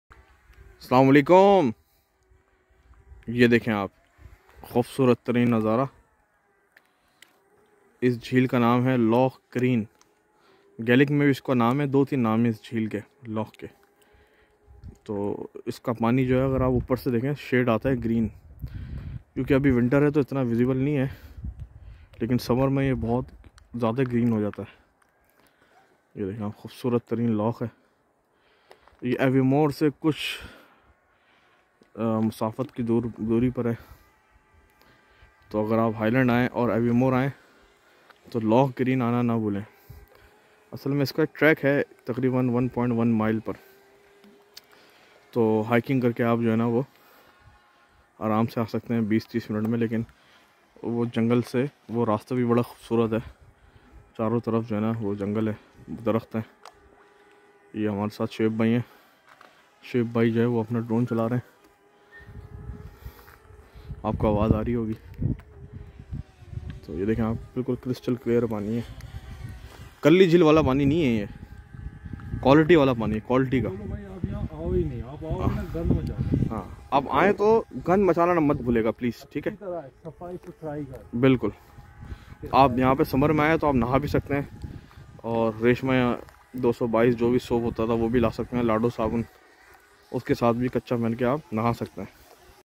Pakistani visited Green Loch!
Next to Loch Morlich few mints away by drive you can counter with this beautiful creation of God. Its stunning and peaceful!